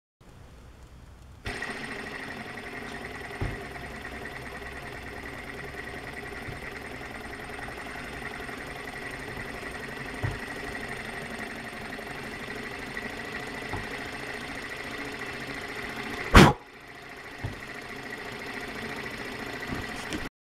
Sunnytech Stirling Engine Generator (Model SC011)
Kicks off with a flick of the finger after about 30 seconds heating with the denatured alcohol (ethanol) flame.

See the Amazon listing for details: